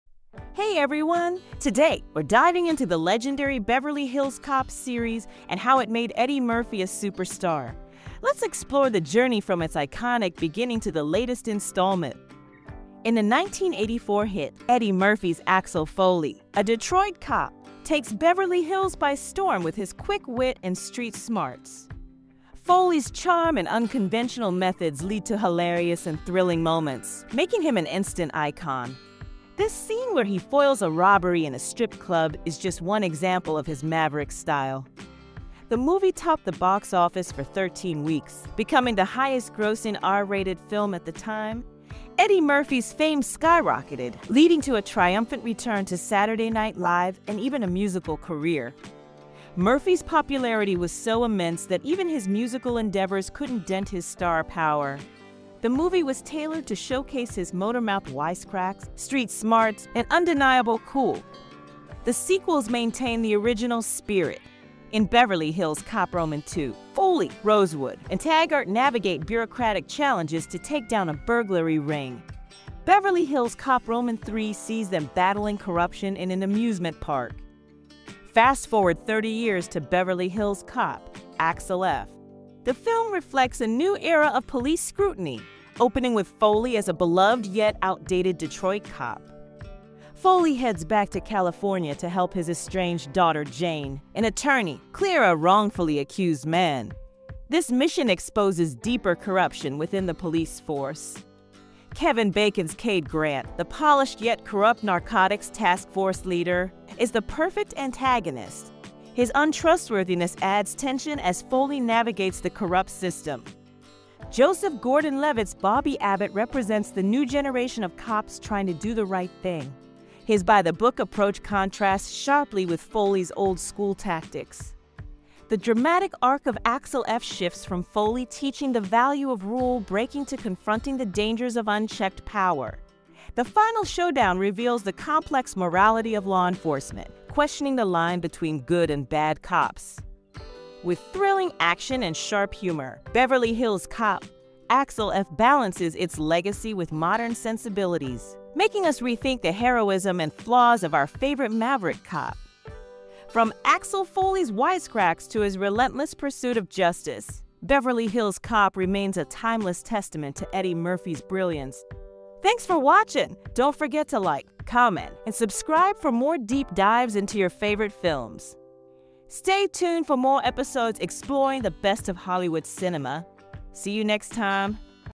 'Beverly Hills Cop Axel F' Is an Enjoyable Retread
Scientists Discover Simple
‘Tea Hack’ To Help Boost Metabolism And Release
Belly Fat:

 legendary Beverly Hills Cop series that skyrocketed Eddie Murphy to stardom. Starting with the 1984 hit, Eddie Murphy's Axel Foley, a street-smart Detroit cop, takes Beverly Hills by storm with his quick wit and unconventional methods, creating iconic and hilarious moments. The film topped the box office for 13 weeks, cementing Murphy's fame and leading to sequels that retained the original's spirit. Fast forward to Beverly Hills Cop: Axel F, we see Foley navigate a corrupt system to help his estranged daughter, with new characters like Kevin Bacon's corrupt Cade Grant and Joseph Gordon-Levitt's by-the-book Bobby Abbott adding tension. The latest installment balances action and humor while exploring modern themes of police scrutiny and morality. Beverly Hills Cop remains a testament to Eddie Murphy's brilliance, blending thrilling action with sharp humor.